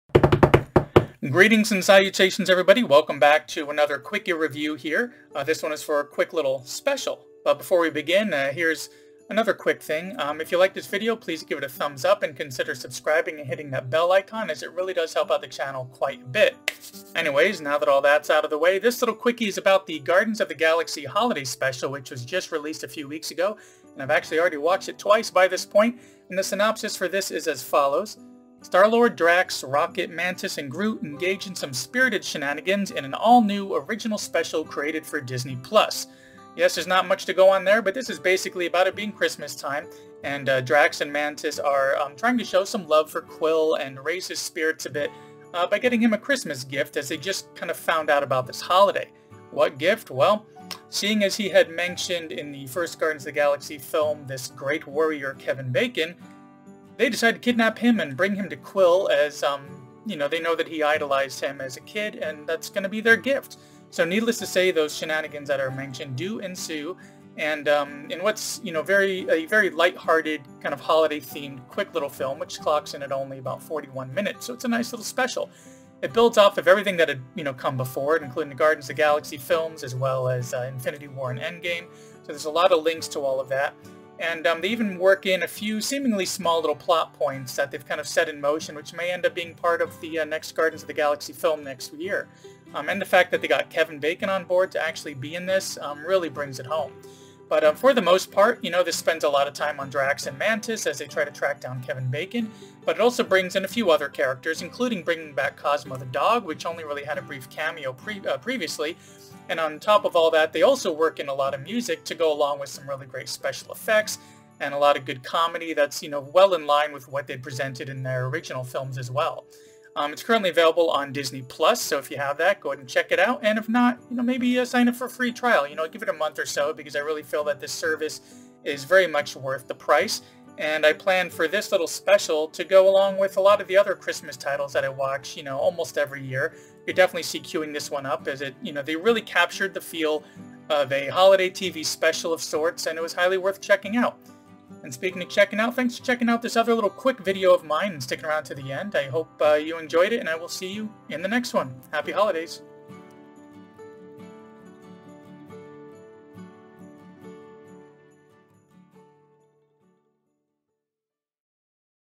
Guardians of the Galaxy Holiday Special (2022) - Non-Spoiler Quickie Review
Released on November 25th 2022, they really captured the look and feel of a television Holiday Special.

Synopsis:
Star-Lord, Drax, Rocket, Mantis, and Groot engage in some spirited shenanigans in an all-new original special created for Disney+.